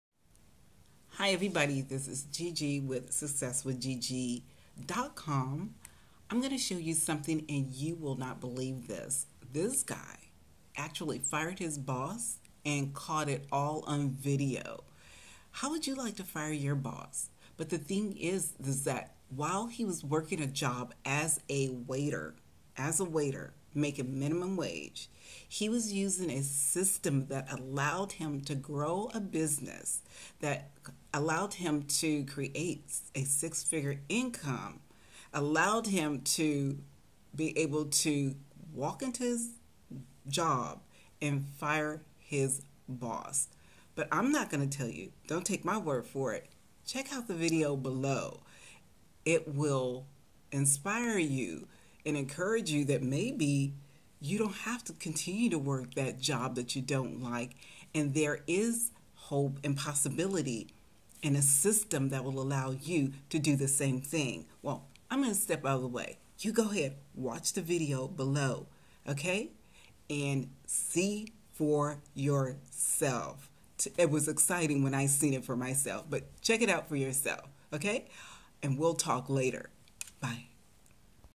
He Physically Fired His Boss On Video
- watch video as this guy walks into his job and fired his boss. All because he used a system that allowed him to grow his home business while working his job.